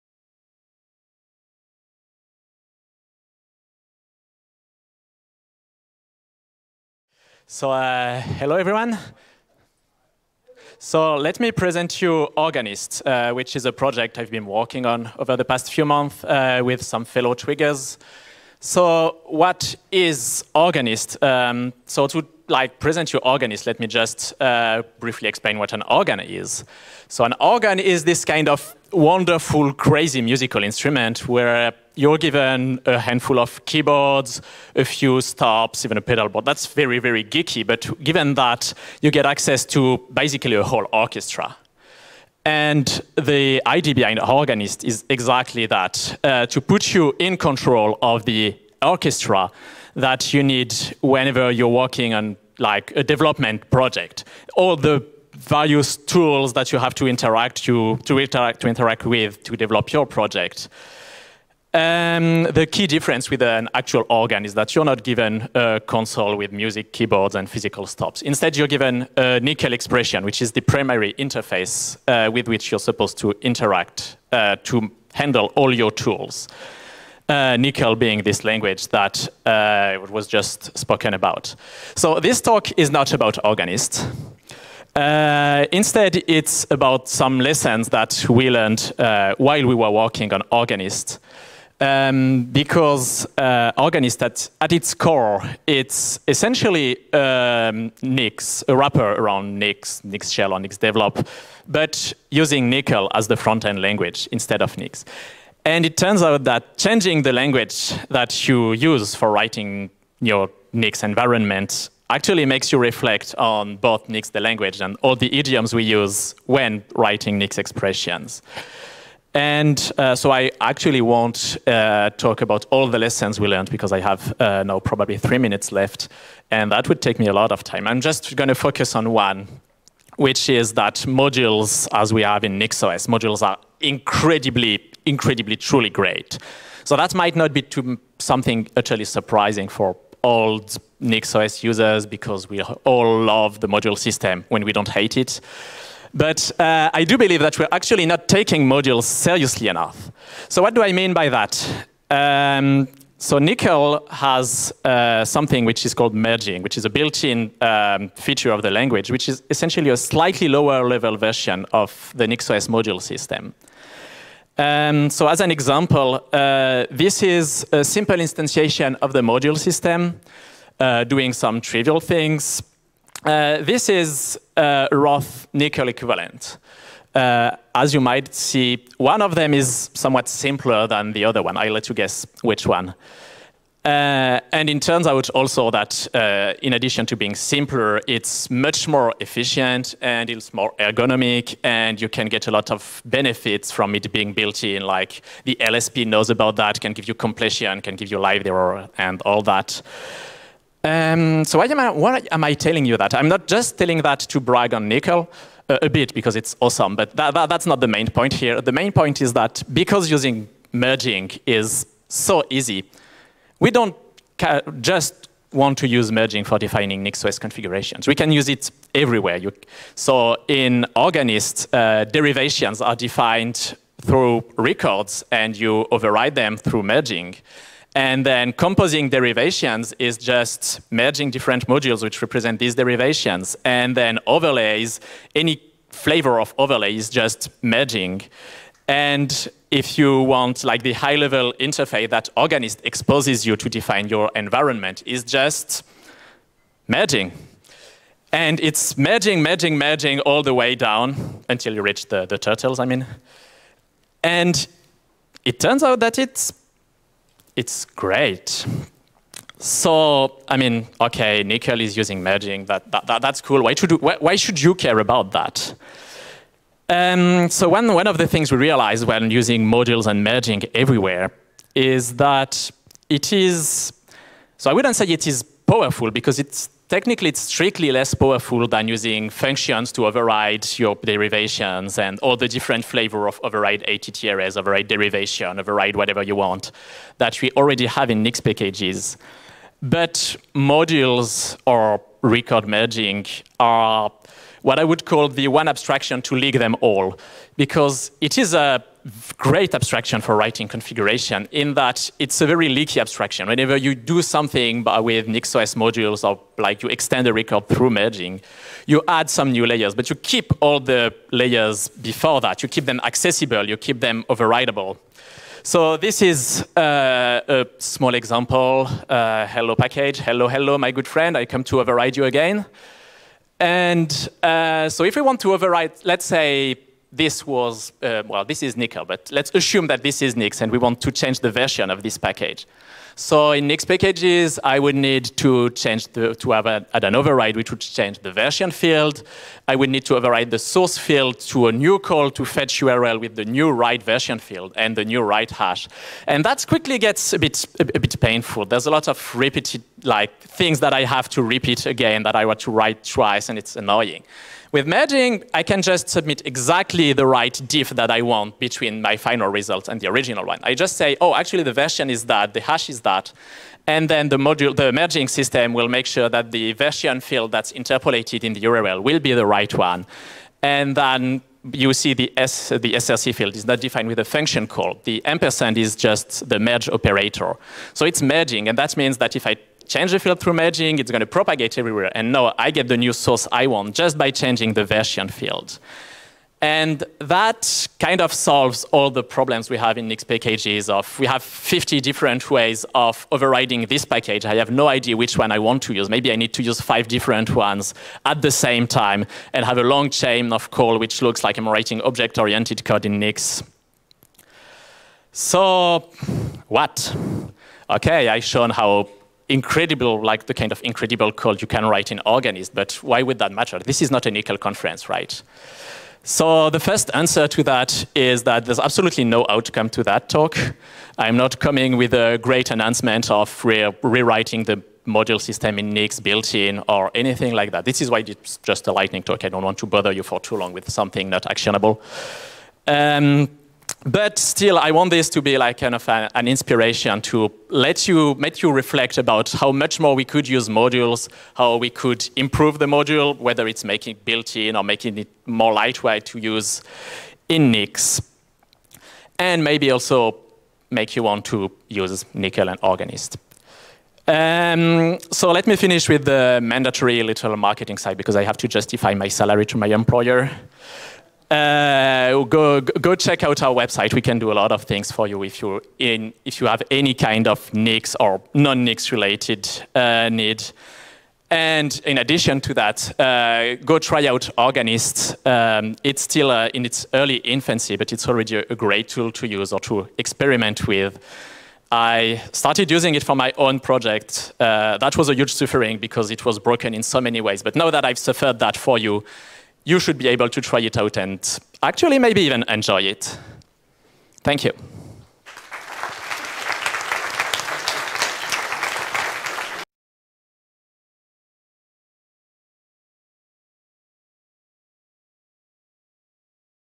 NixCon2023 Nix, but with a different frontend
This talk hints at how defining Nix packages (and development shells in particular) with another language than Nix influences the way we think about them, and discuss whether there are lessons to be learned from here for Nixpkgs or Nix itself.

[Organist] is a Nickel library and tool aimed at serving as the entry point for all the development tools, and featuring in particular a [Nickel] frontend for Nix as an experiment in changing the surface language.
Changing the frontend used was also an occasion of changing the way we declare, compose and override packages, with a framework based on merging records (_a la_ module-system) rather than stacked wrapping functions and explicit fixpoints.

Théophane Hufschmitt